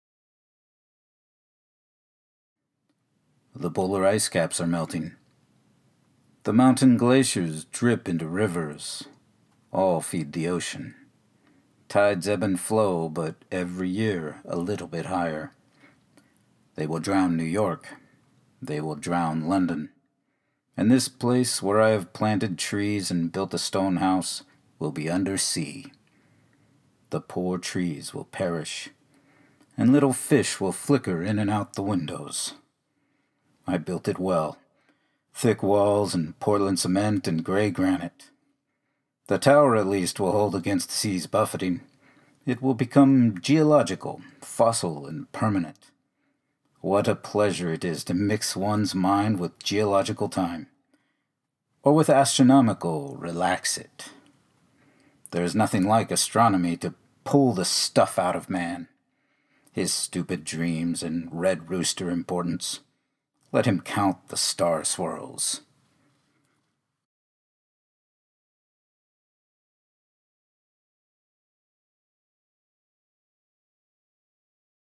The Polar Ice Caps ..., by Robinson Jeffers
Back around 1960, the old poet in the tower had a word to say about global warming and flooding, but never fear: he wasn't trying to frighten you. He was trying to calm you.

Your support for Tor House is appreciated.

Published posthumously.
Jeffers Literary Properties
Stanford University Press